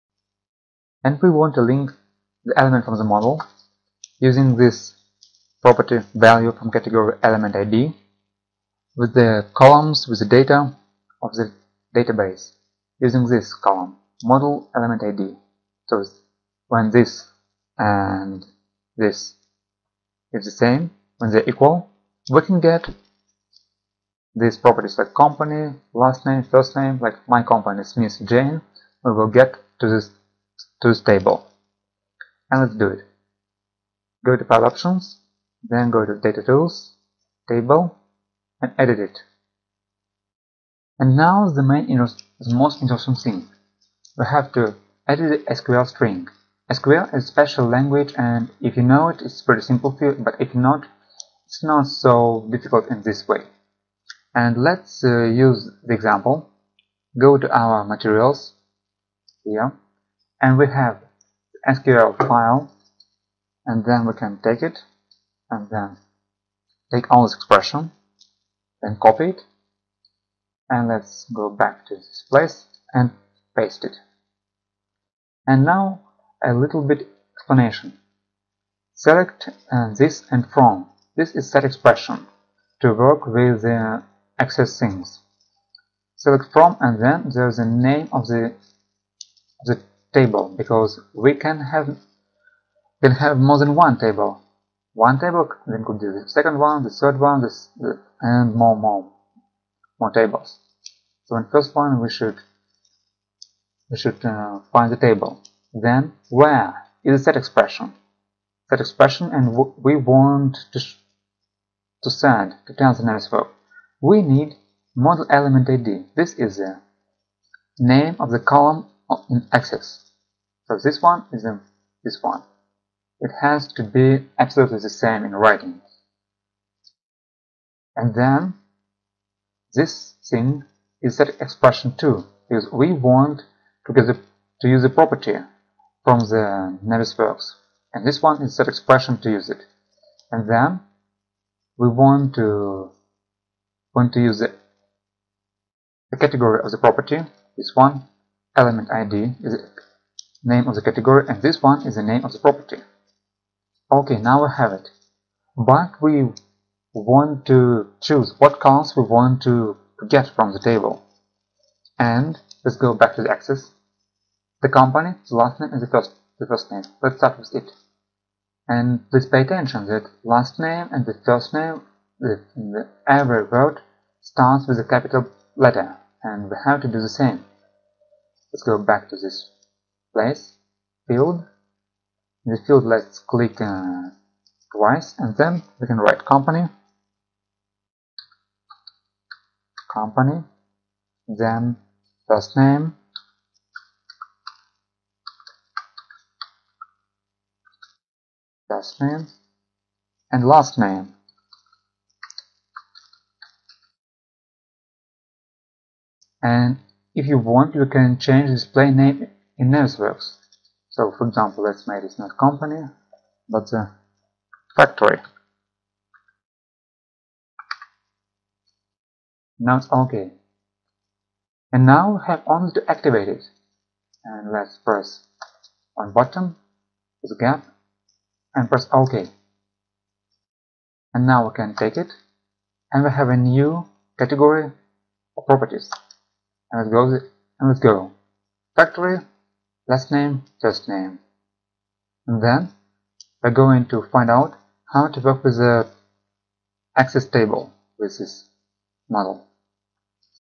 VC: Autodesk Navisworks: 10.03. SQL
To link database and model elements you should chose mapping parameter. In this video, you will make the instruction to Navisworks, how to link database and model.